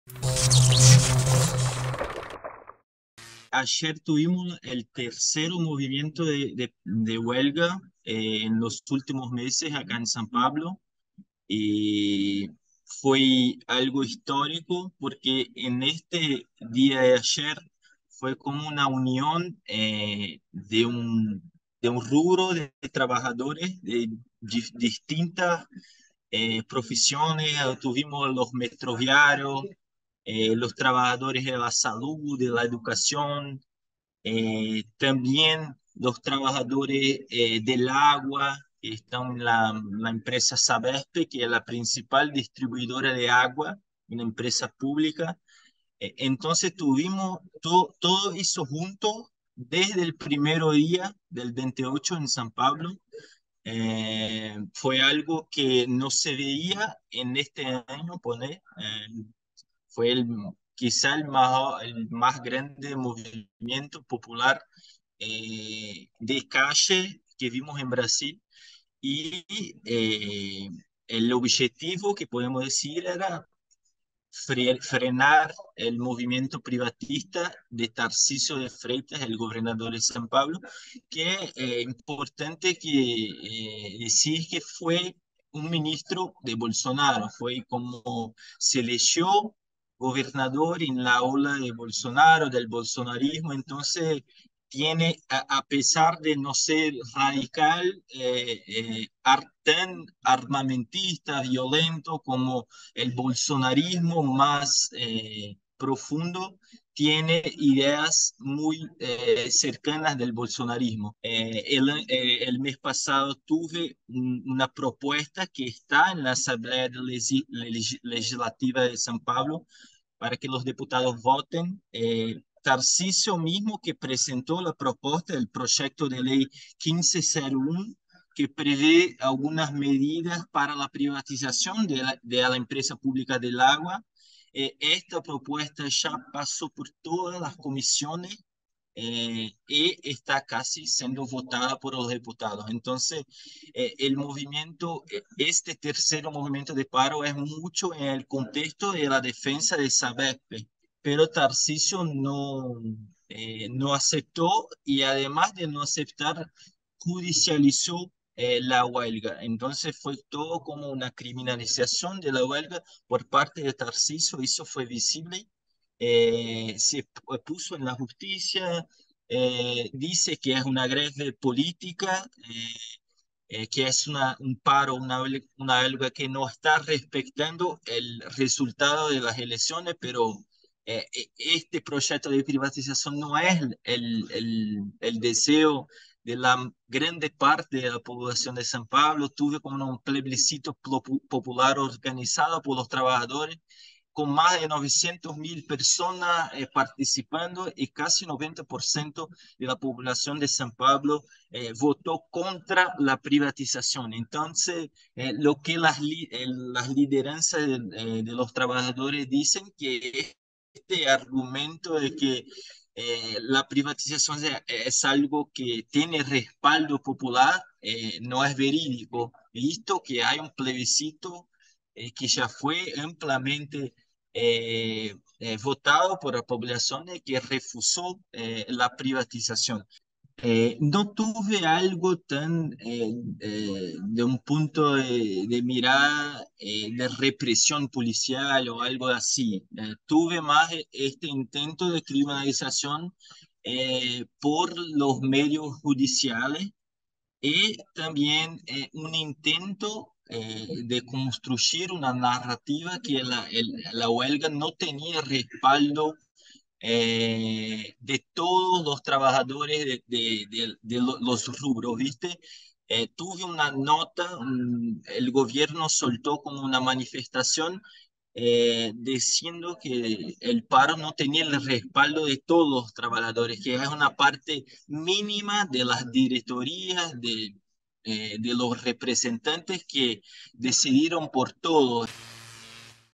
Huelga general en San Pablo contra el plan de privatizaciones
El gobernador bolsonarista Tarcisio de Freitas busca avanzar sobre los servicios públicos de la ciudad más poblada de Brasil. Trabajadores de distintos sectores respondieron con un paro y una masiva movilización en las calles. "El gobierno judicializó la protesta", dijo a Barricada TV Pedro Stropasolas, comunicador del medio popular Brasil de Fato.

Sumate a la Comunidad BTV y ayudanos a sostener esta pantalla alternativa